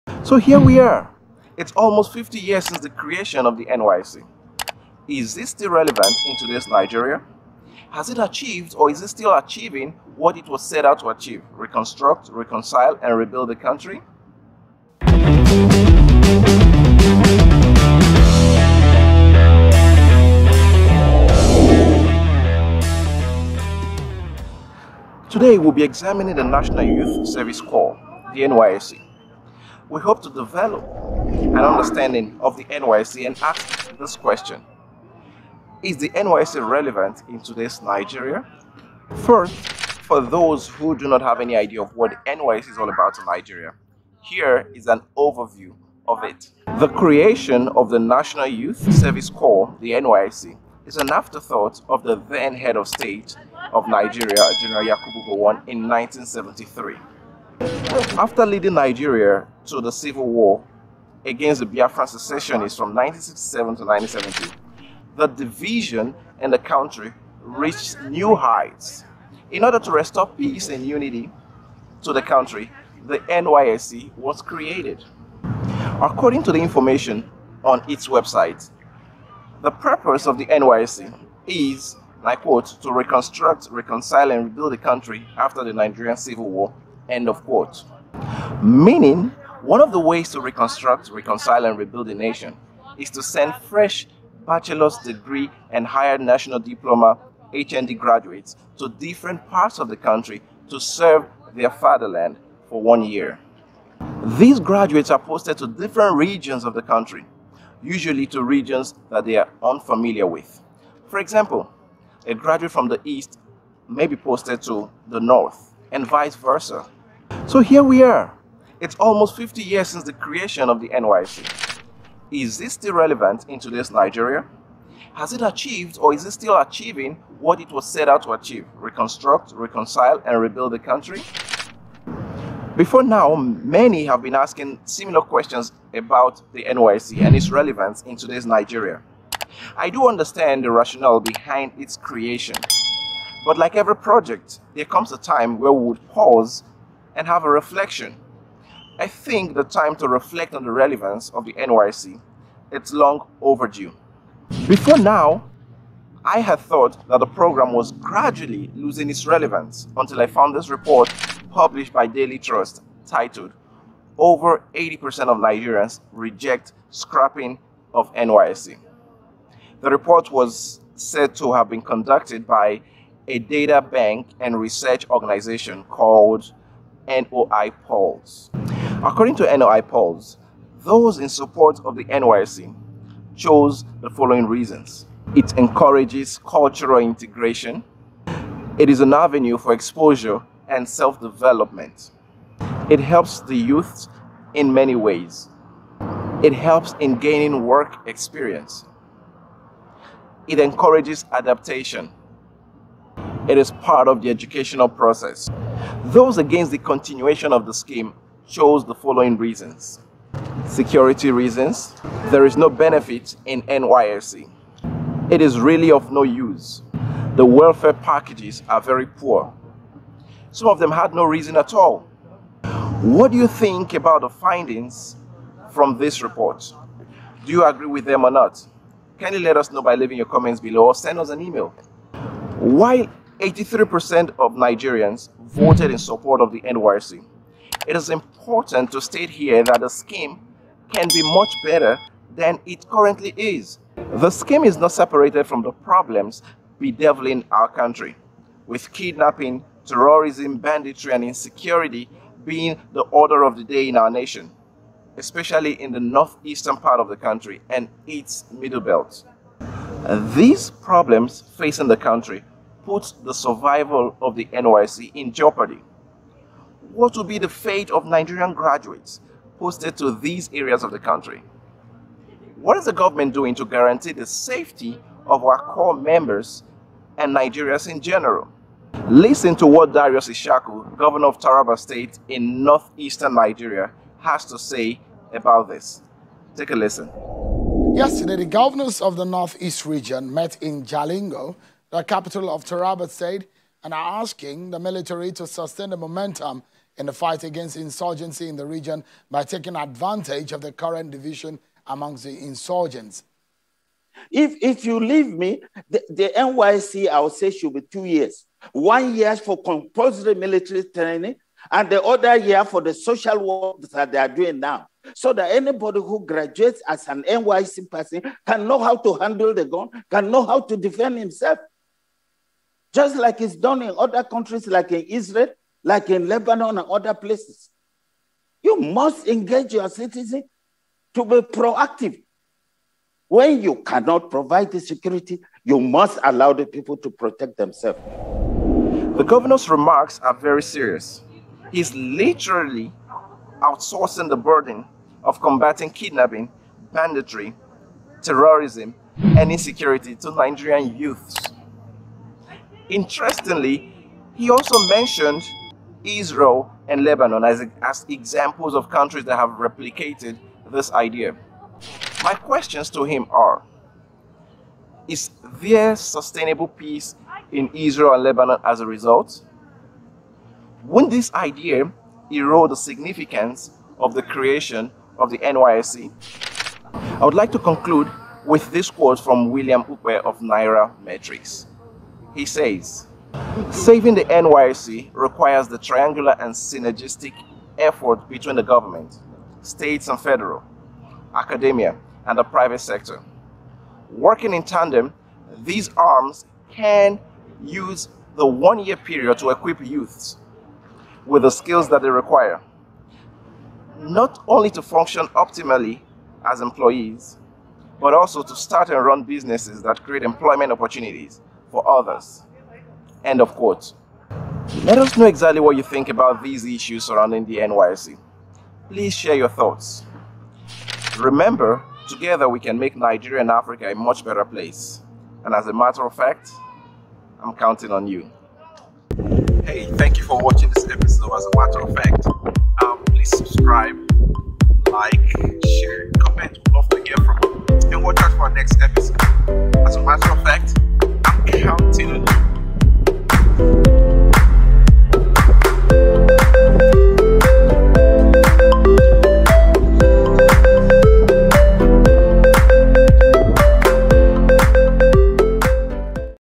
NYSC: To Be Or Not To Be?
NYSC: To Be Or Not To Be? | As A Matter Of Fact, | Episode 8

There has been a recent debate about the relevance of the National Youth Service Corps (NYSC) in today's Nigeria. This episode provides the history behind its creation, the challenges it faces, and the way forward.

Share your thoughts.